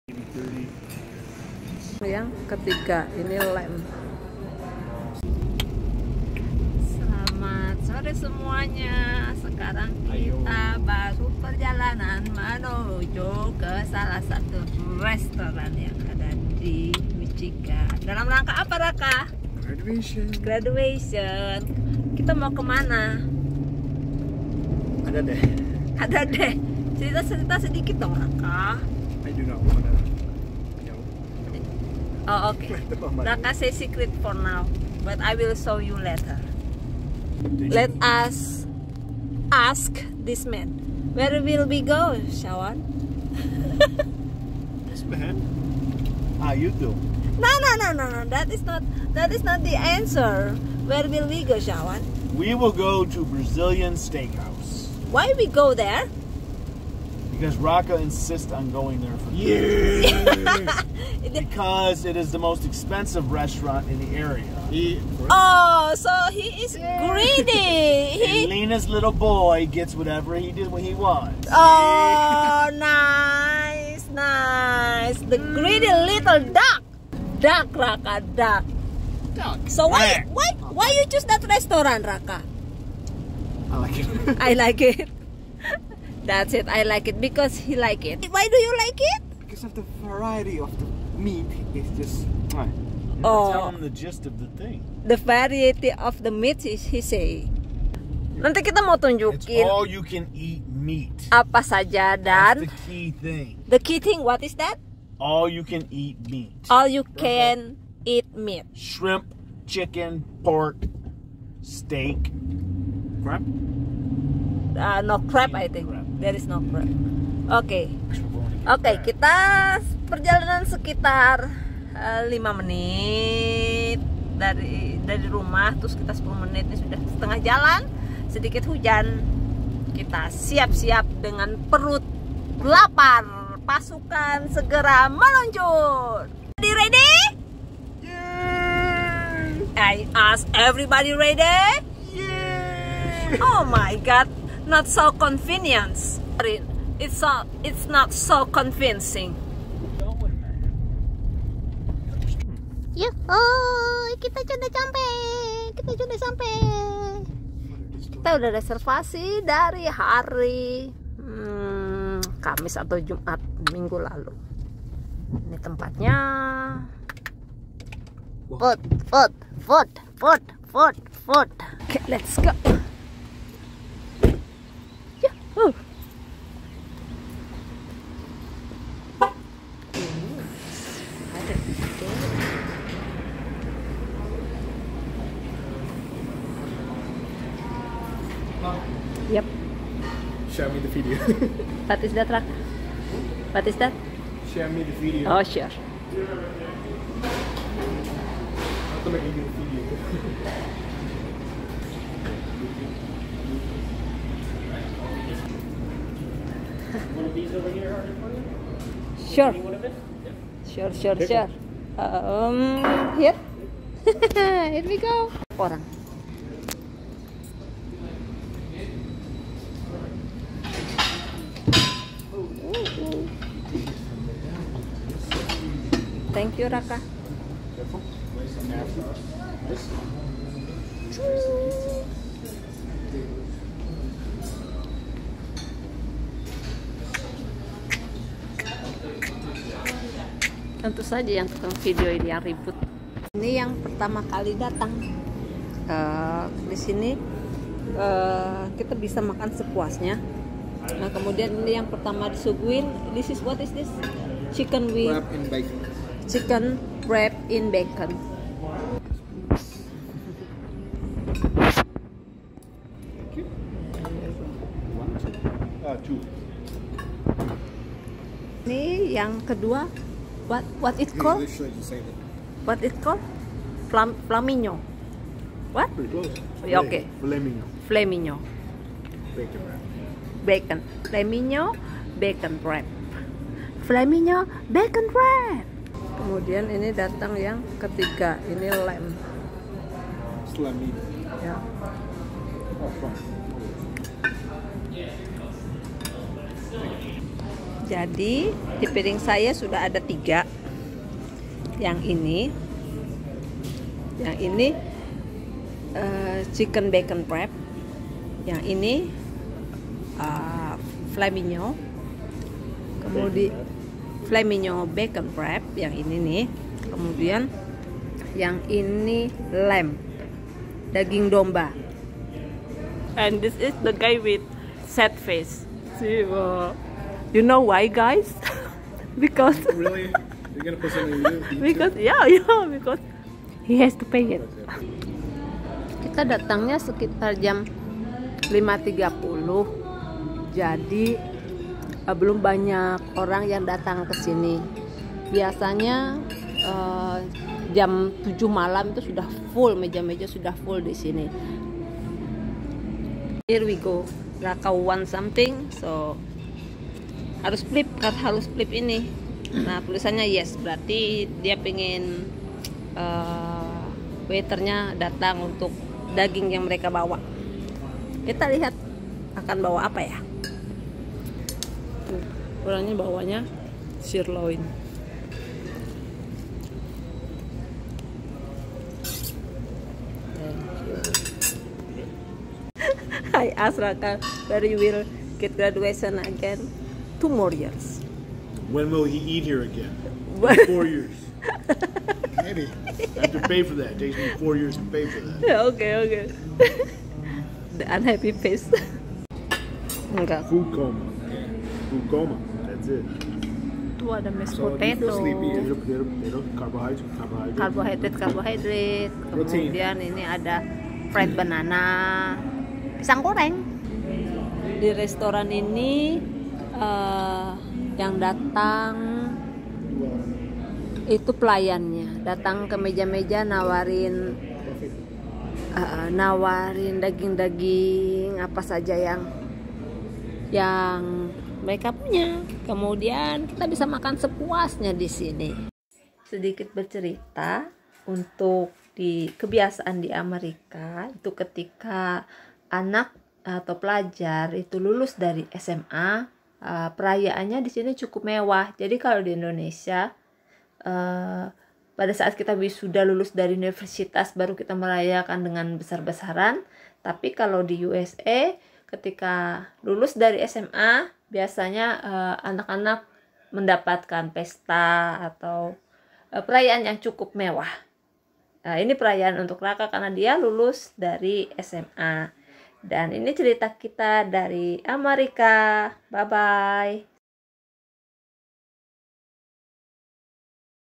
Perayaan Graduation SMU Raka di Brazilian steakhouse house.
Merayakan kelulusan Raka dr salah satu SMA di Michigan. Di USA perayaan lulus SMA sama meriah dengan perayaan kelulusan kuliah di Indonesia